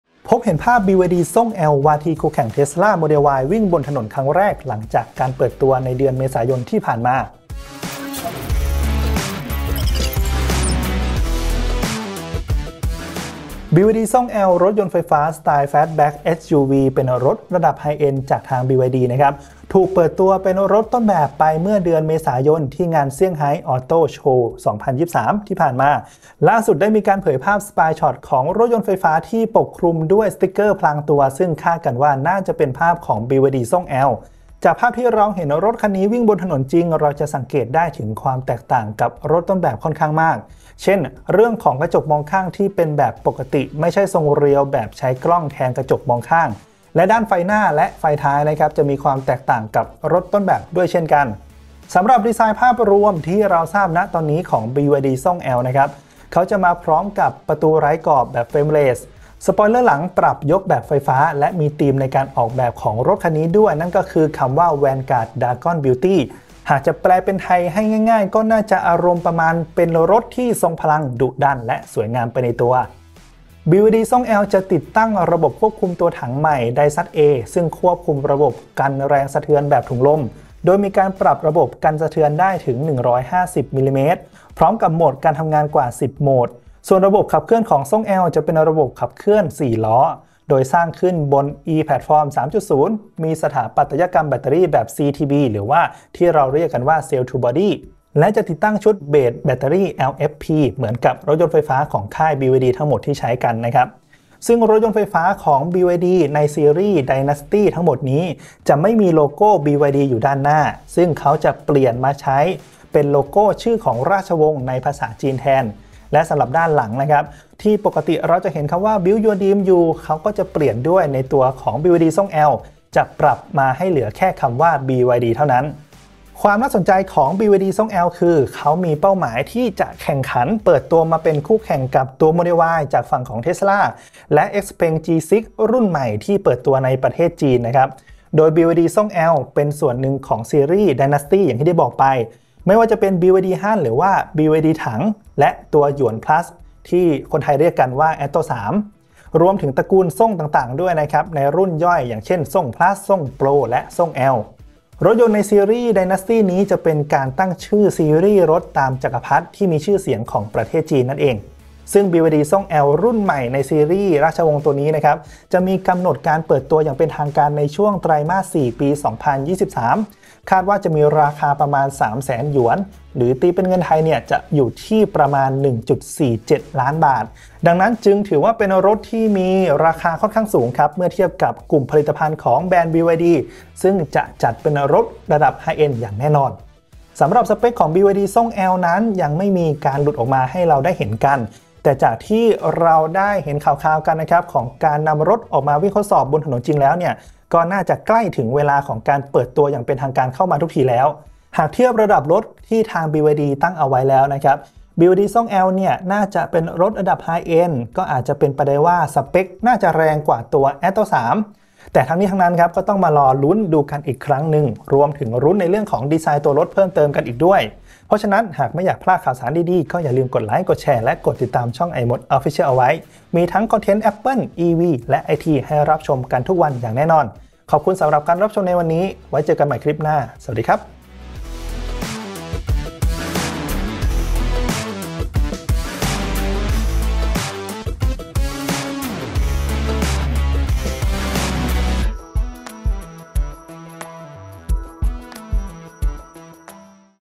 พบ BYD Song L ว่าที่คู่แข่ง Tesla Model Y วิ่งบนถนนครั้งแรก!
🔴 BYD Song L รถยนต์ไฟฟ้าสไตล์ Fastback SUV เปิดตัวเป็นรถต้นแบบไปเมื่อเดือนเมษายน ที่งาน Shanghai Auto Show 2023 ที่ผ่านมา ล่าสุดได้มีการเผยภาพ Spyshot รถยนต์ไฟฟ้าที่ปกคลุมด้วยสติกเกอร์พรางตัว ซึ่งคาดกันว่าน่าจะเป็นภาพของ BYD Song L

รายละเอียดเพิ่มเติม :
ภาพ Spyshot BYD Song L รถยนต์ไฟฟ้า SUV ถูกพบเห็นบนถนนครั้งแรก : ​